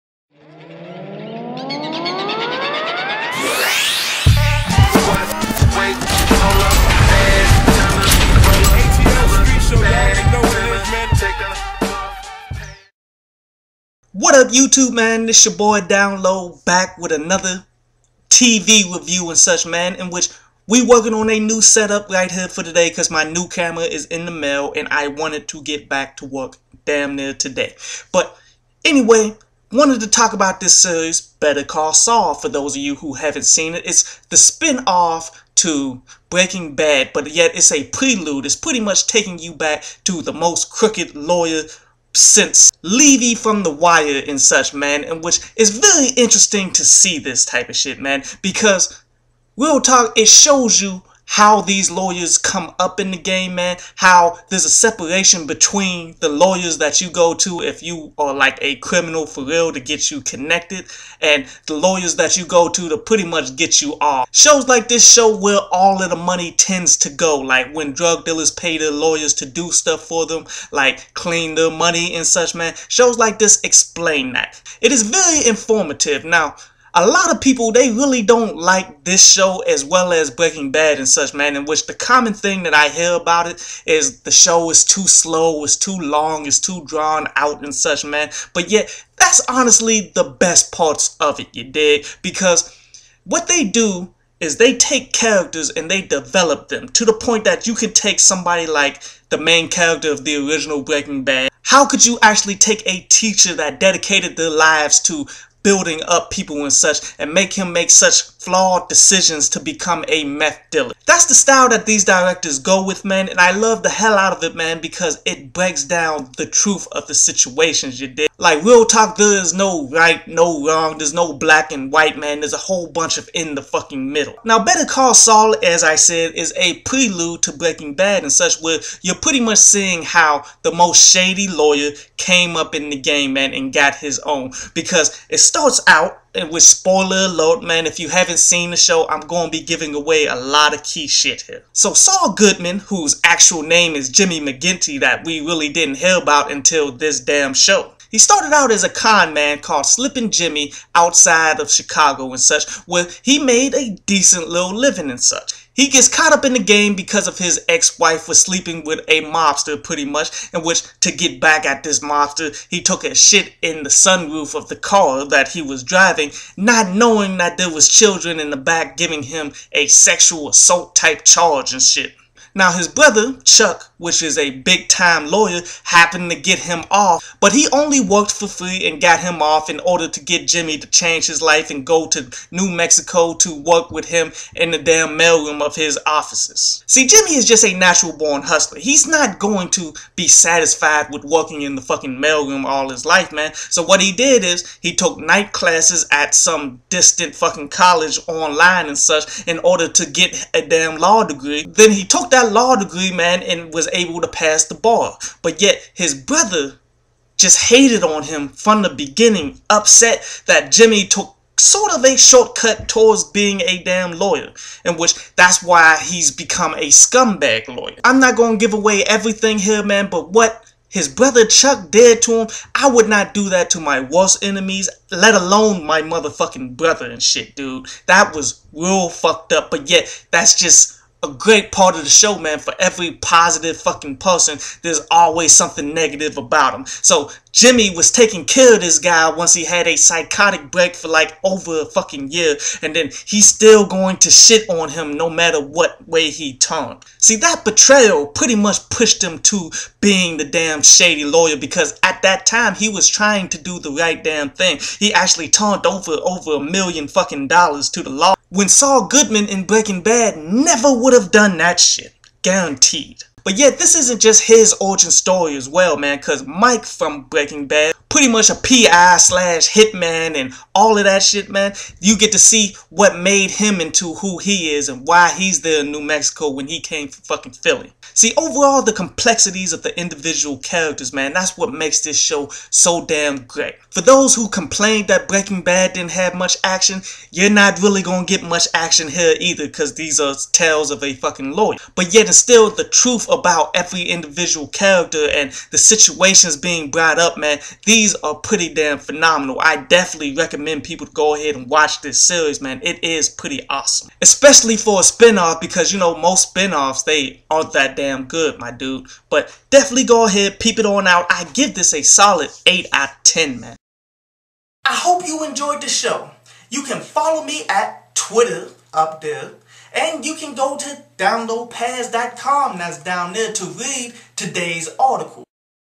Better Call Saul (Season 1 Review)
This is a review for Season 1 of Better Call Saul, spin off for Breaking Bad. This is a very informative show about the differences in lawyers, how lawyers get connected, how they launder money, and more about the scumbag lawyer profession. Any fan of Breaking Bad would like this as we watch the origin story of fan favorites Saul Goodman and Mike. 8/10